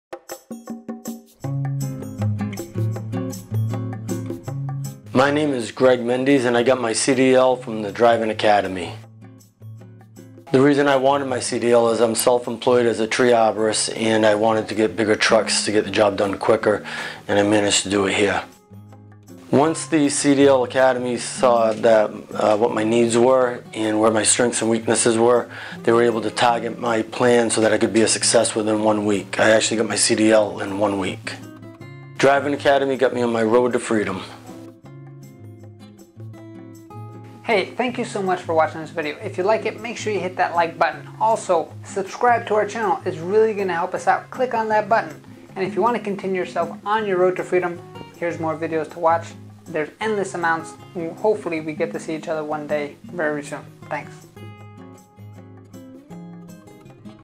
Greg Passed His CDL Road Test and Earned his CDL! - Driving Academy Student Testimonial
Greg, a student of Driving Academy, earned his CDL by passing the road test! Congratulations on getting on your ROAD TO FREEDOM!

200 East Edgar Road, Linden NJ